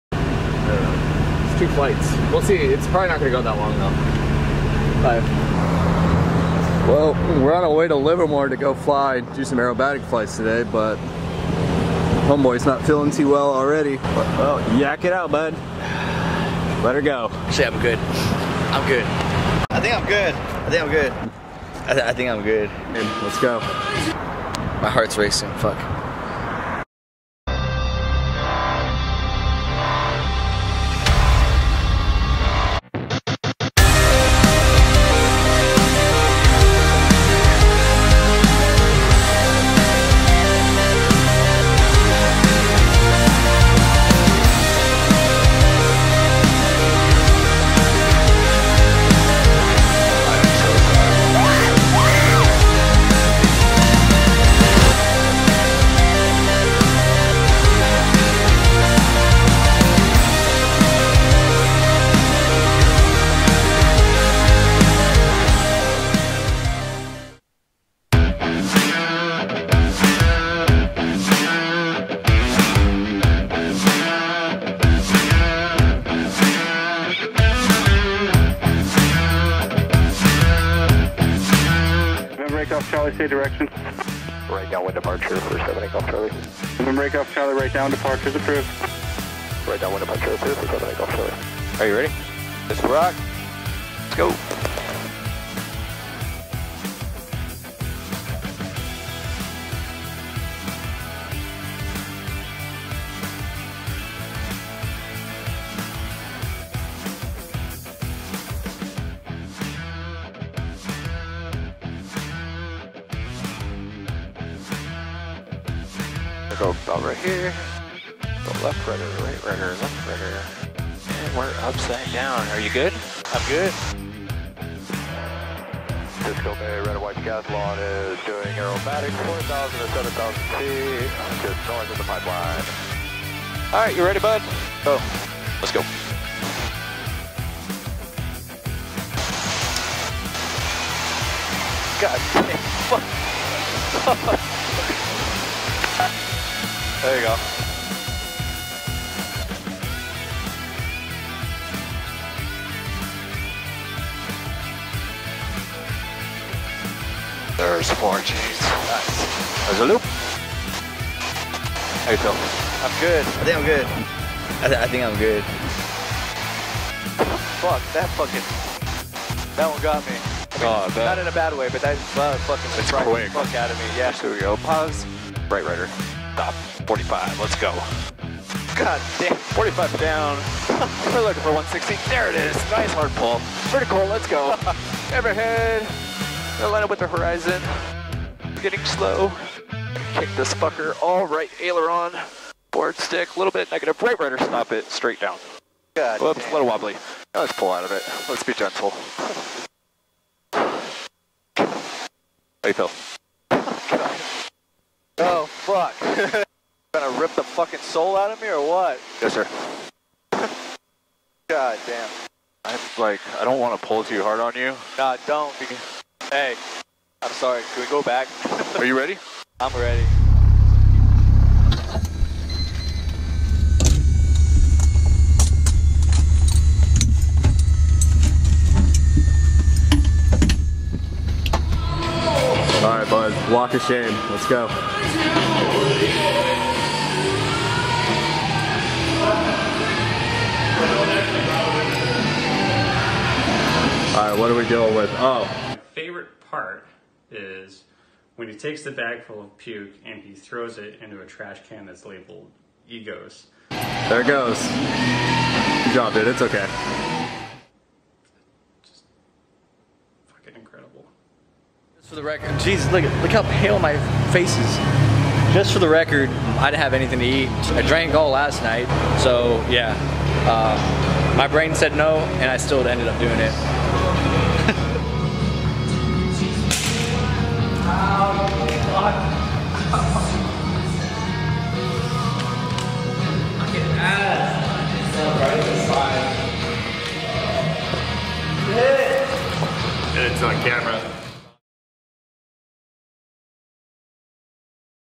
Aerobatic Flight With G force Reactions. PASSENGER GETS SICK!
Huge shoutout to Kevin for going on this last minute aerobatic flight. Unfortunately Kevin wasn't feeling 100% before the flight and still went anyways.

Instagram Accounts
Pilot (me) @josh_invertedaviation / @Inverted_aviation